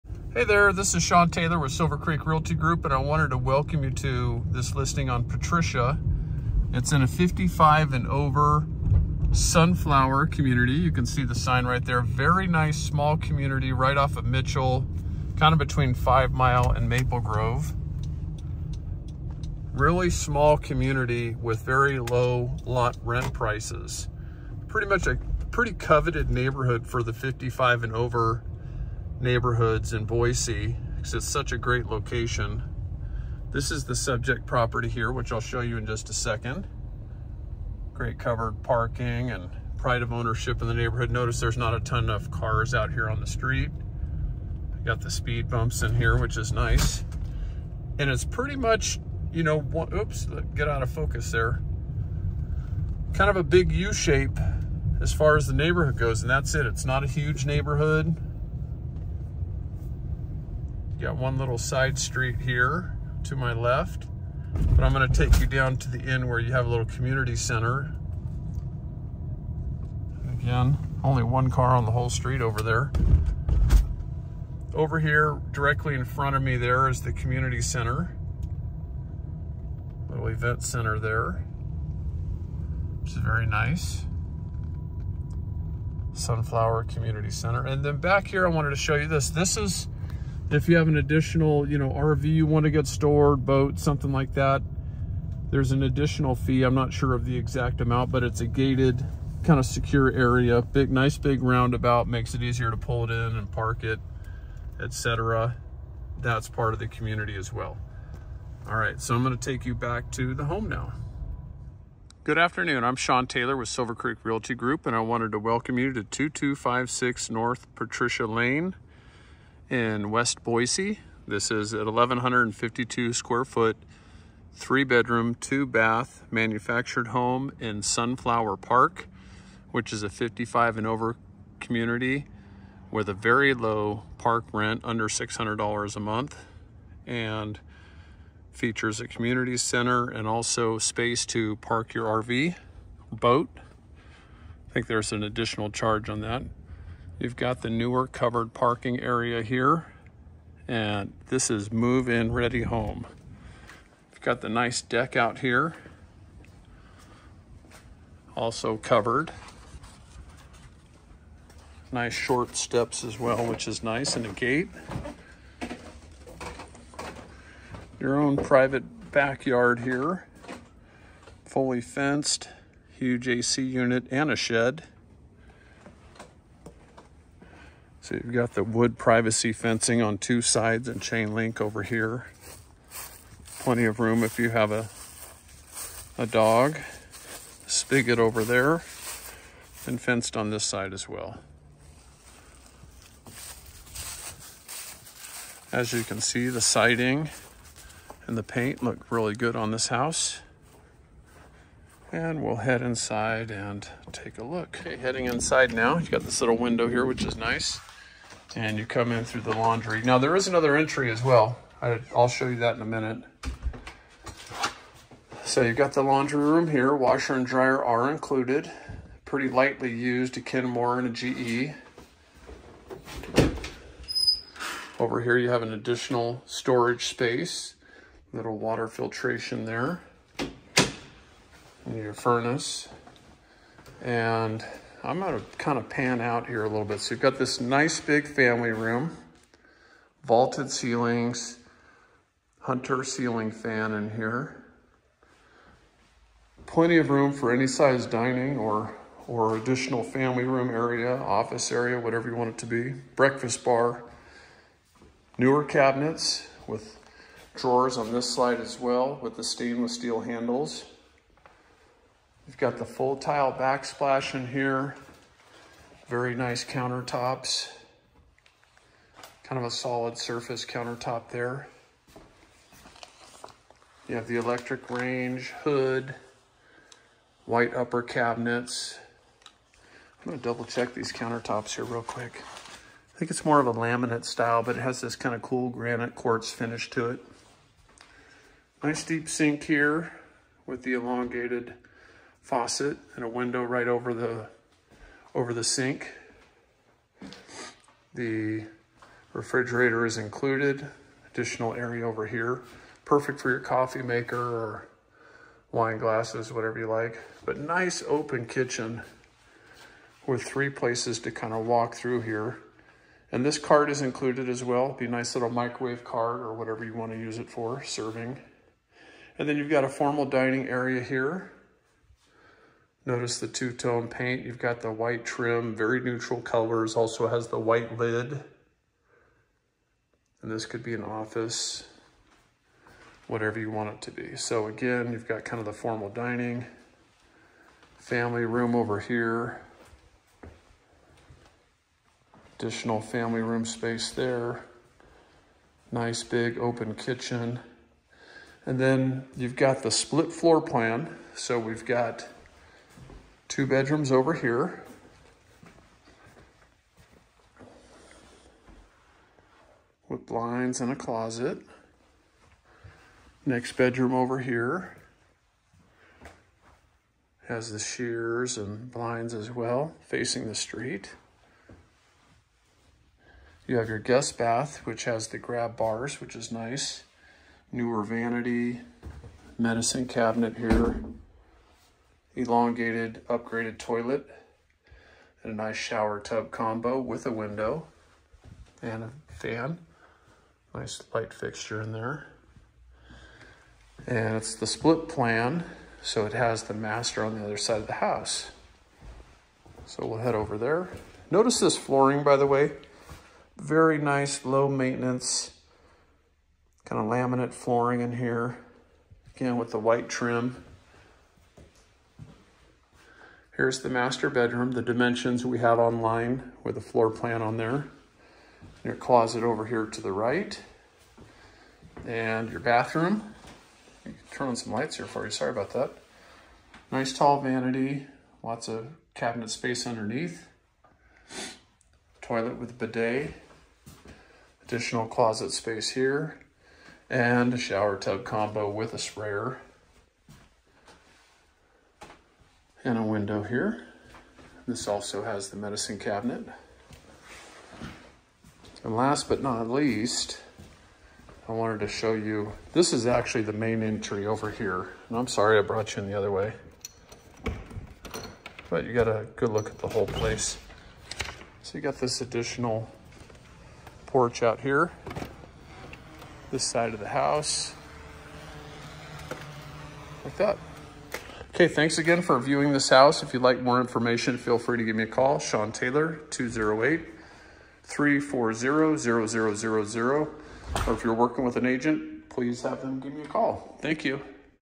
55+ Community! MOVE IN READY HOME!
Move in ready Manufactured home in the coveted West Boise "Sunflower Park" 55+ community. Great location and low lot rent. 3/2 w/spacious living and formal dining room which can also be used as Den or office. Split plan with Master on one side of the home and 2 beds on the other!